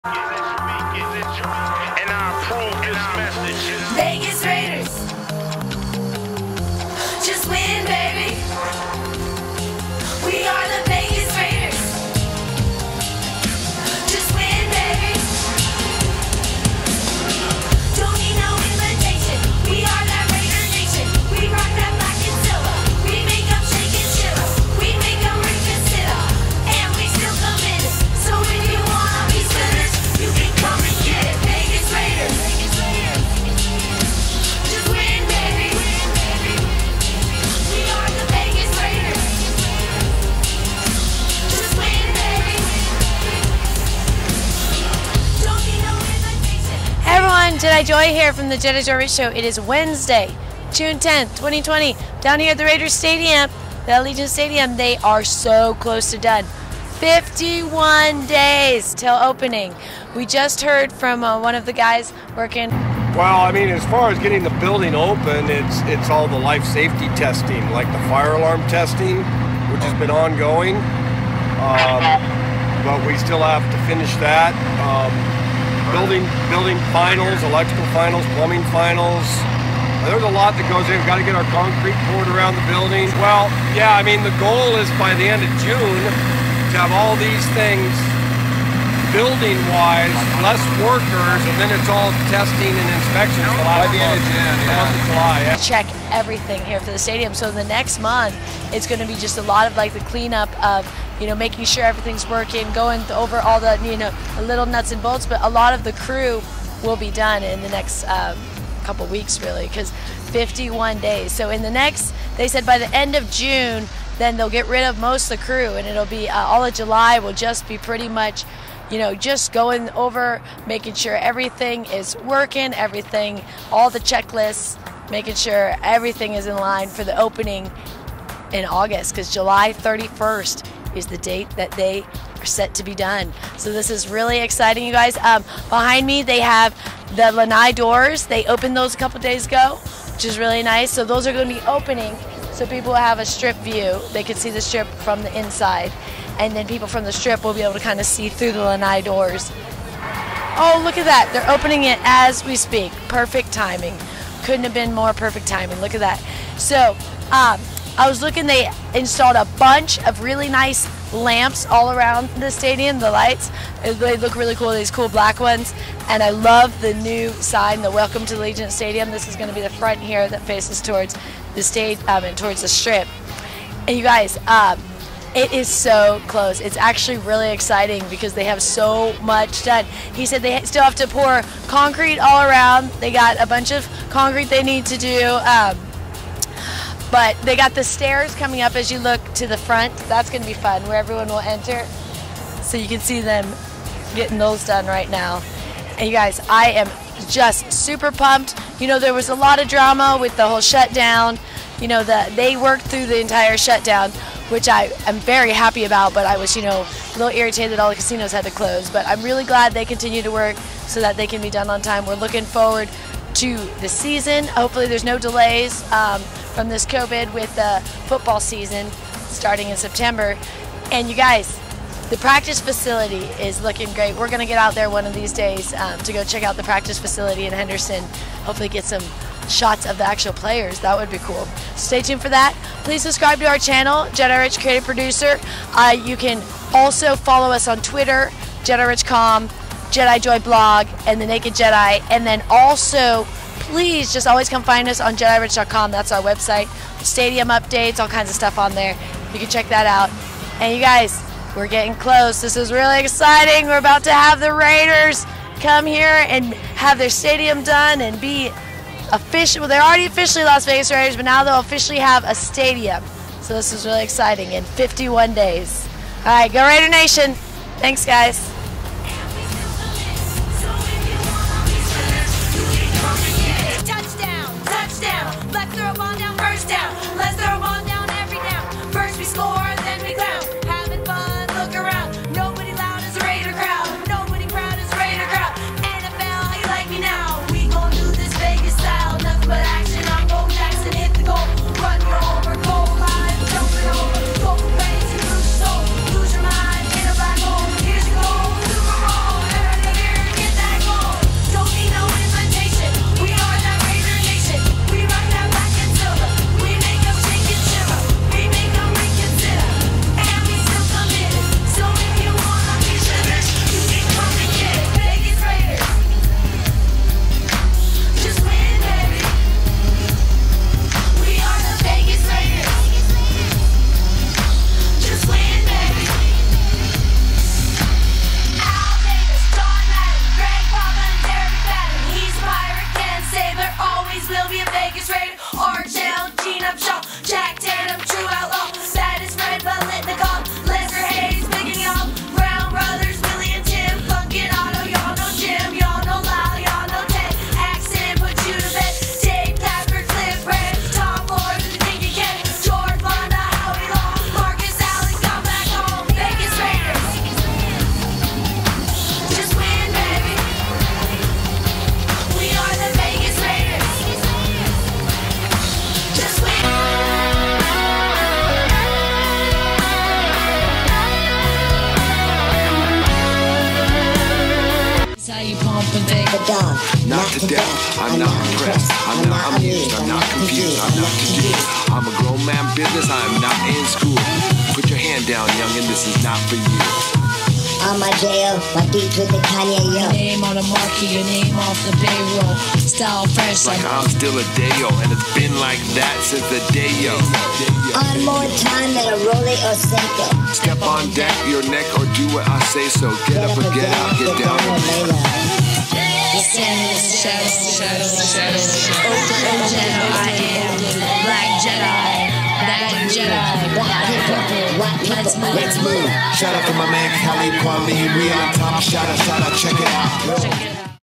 Allegiant Stadium Update: Lanai Doors Opening - Most work willbe done by end of June 2020
Hey Raider Nation! We went to the stadium this morning to check on the progress.  It's looking more gorgeous everyday.  While we were reporting, they opened one of the Lania Doors.  You can see how it moves in the video.  The lockdown is over & the casinos are open! Its time to book your tickets and hotel rooms while the rates are cheap!

Quit consuming: Dairy. Sugar. Alcohol. Caffeine. Gluten. Cocaine. Grains. GMOs. Artificial Sweeteners. Studios